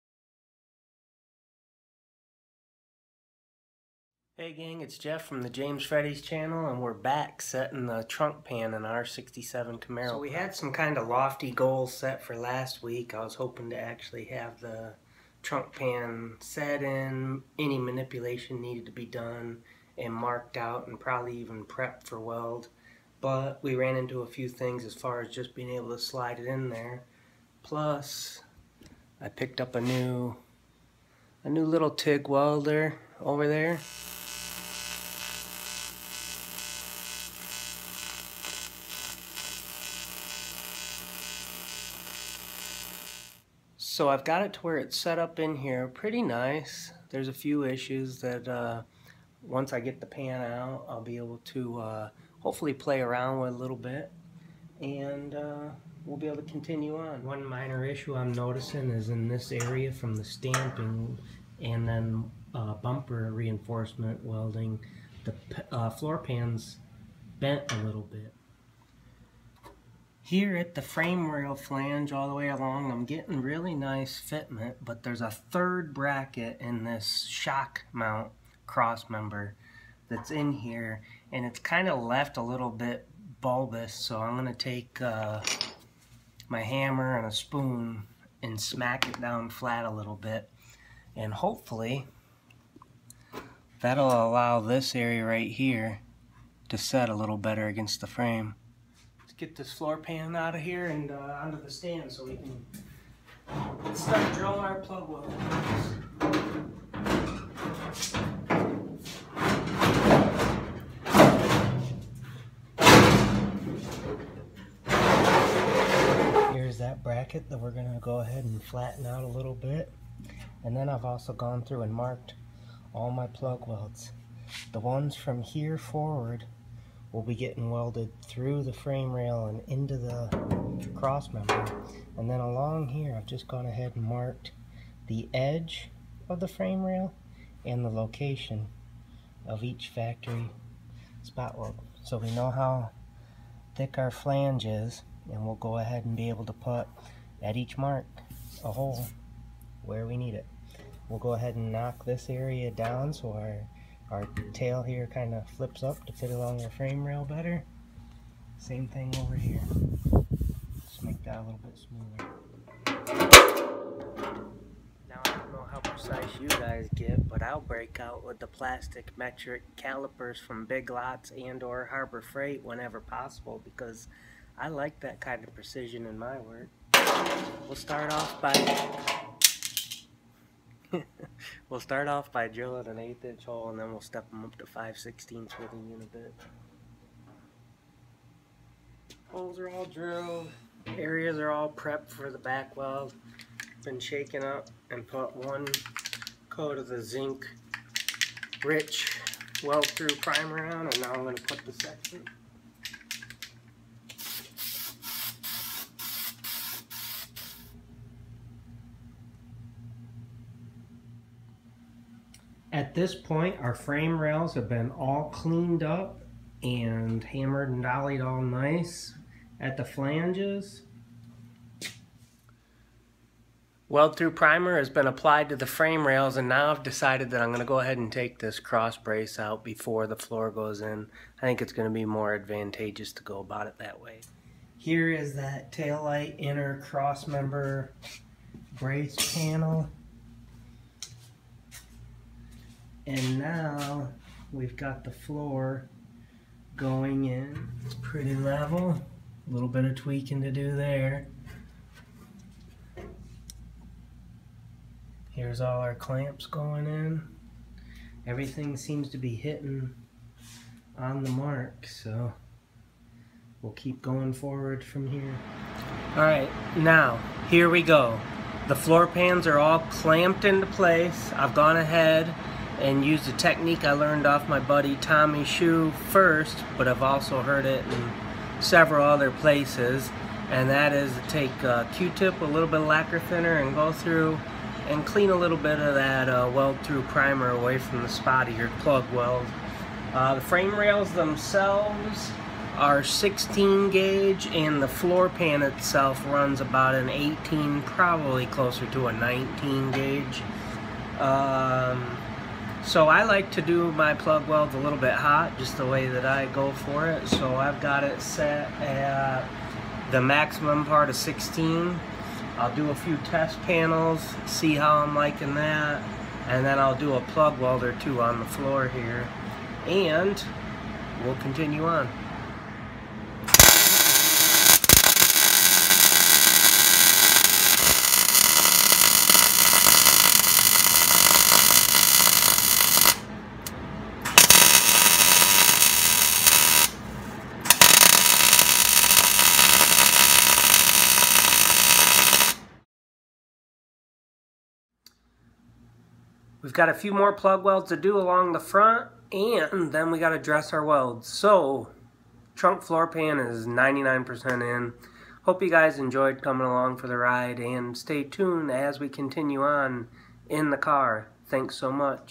1967 Camaro Trunk Pan Installation JAMESFREDDYS MC&HR
Final fittment and installation of the trunk pan in the first generation Camaro.  Follow the Playlist as we build the sheetmetal for our Penske camaro clone.